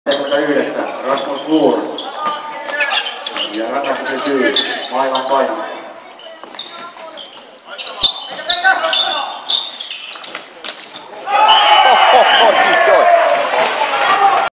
PSS - SB Welhot 2-2
Welhojen tasoitusmaali 2-2:een ylivoimalla ensimmäisessä erässä: Taskinen (Lankinen)